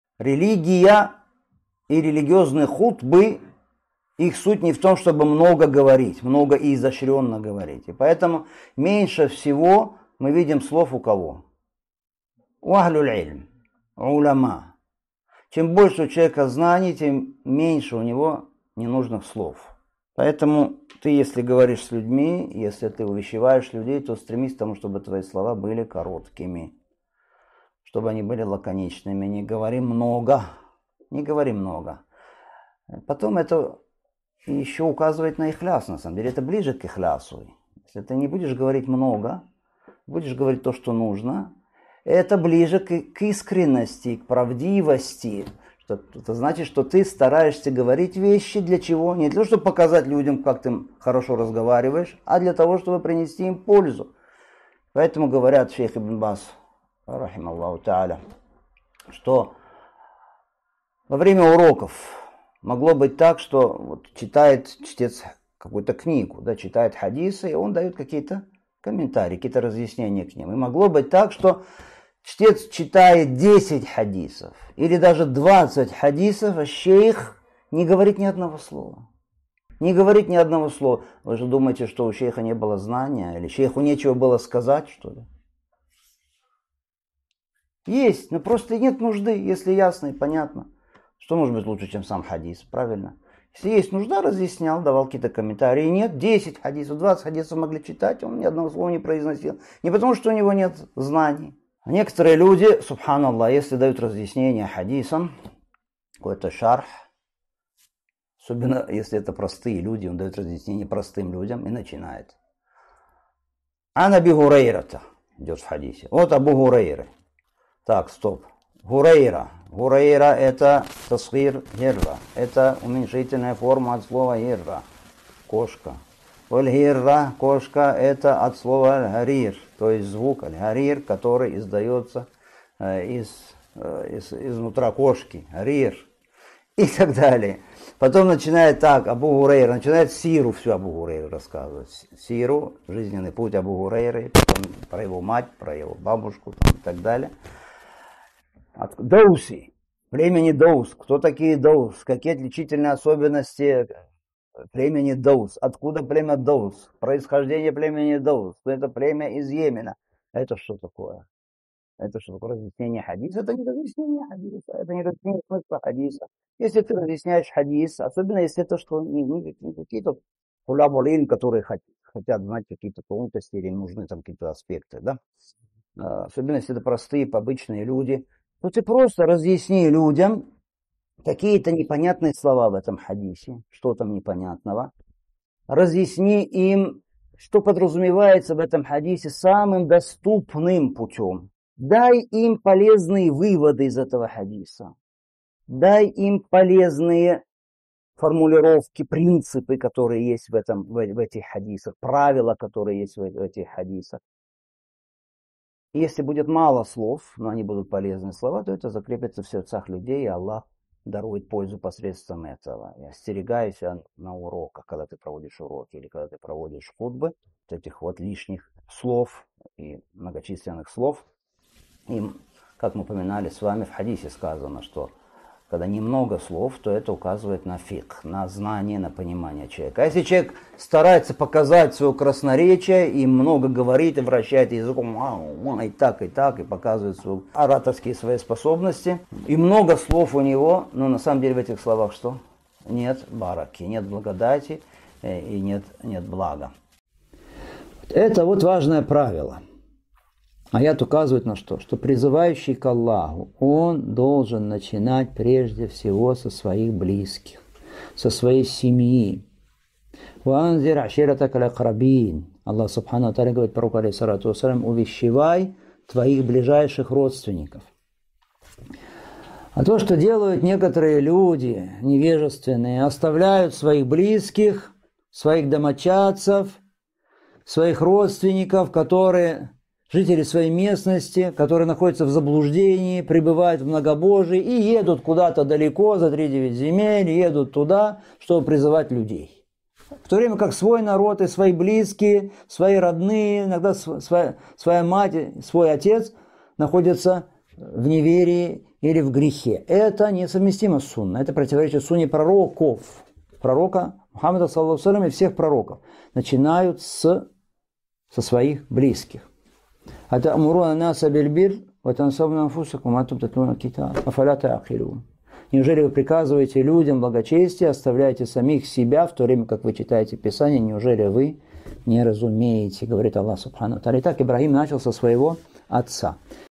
Чем больше знания, тем меньше слов! | Пользы из Сахиха Муслима. Абу Яхья Крымский
Целая лекций на Google Диск: 8 Сахих Муслима. Книга о пятничной молитве

Суть религии и религиозных проповедей (хутб) не в том, чтобы много и изощрённо говорить. И поэтому меньше всего слов мы видим у учёных (ахлю ль-'ильм, 'уляма). Чем больше у человека знаний, тем меньше у него ненужных слов. Поэтому если ты говоришь с людьми, увещеваешь их, то стремись, чтобы твои слова были короткими, лаконичными, не говори много.

Потом, это ещё указывает на искренность (ихляс), на самом деле, это ближе к ихлясу. Если ты не будешь говорить много, будешь говорить то, что нужно, — это ближе к искренности и правдивости, это значит, что ты стараешься говорить вещи не для того, чтобы показать людям, как ты хорошо разговариваешь, а для того, чтобы принести им пользу.

Во время уроков шейха Ибн База (да помилует его Всевышний Аллах) чтец читал какую-то книгу, хадисы и он (Ибн Баз) давал какие-то комментарии, разъяснения. И могло быть так, что чтец читает десять даже двадцать хадисов и шейх не говорит ни одного слова. Вы же думаете, что у шейха не было знания или шейху нечего было сказать, что ли? Было, но просто не было нужды, если ясно и понятно. Что может быть лучше, чем сам хадис, правильно? Если была нужда — разъяснял, давал какие-то комментарии, нет — десять или двадцать хадисов могли читать, а он ни одного слова не произносил, не потому, что у него не было знания.

Некоторые люди, Пречист Аллах (Субхана Ллаh), если дают какое-то разъяснение (шарх) хадисам, особенно если дают разъяснение простым людям, начинают: «От Абу Хурайры... Так, стоп. "Хурайра" — это уменьшительная форма от слова "хирра" (кошка), а "хирра" (кошка) — это от слова "харир" (звук, который издаётся из нутра кошки)» — и т. д., потом начинает весь жизненный путь (сиру) рассказывать, про его мать, его бабушку и т. д., ад-Давси (из племени давс) — кто такие давс, какие его отличительные особенности, откуда оно, его происхождение: «Это племя из Йемена...» Это что такое?! Разъяснение хадиса?! Это не разъяснение смысла хадиса.

Если ты разъясняешь хадис, особенно если это не какие-то искатели знания (тъуллябу ль-'ильм), которые хотят знать какие-то тонкости или которым нужны какие-то аспекты, если это простые, обычные люди, ты просто разъясни людям какие-то непонятные слова в этом хадисе, что там непонятного, разъясни им, что подразумевается в этом хадисе, самым доступным путём, дай им полезные выводы из этого хадиса, дай полезные формулировки, принципы, правила, которые есть в этих хадисах. Если будет мало слов, но они будут полезные, то это закрепится в сердцах людей и Аллах дарует пользу посредством этого. И остерегайся, когда ты проводишь уроки или хутбы, этих лишних и многочисленных слов.

И, как мы с вами упоминали, в хадисе сказано, что, когда немного слов, это указывает на знание, понимание (фикъх) человека. А если человек старается показать своё красноречие, и много говорит, и вращает языком и так и так, и показывает свои ораторские способности, и у него много слов, то в его словах нет благодати (бараки) и блага.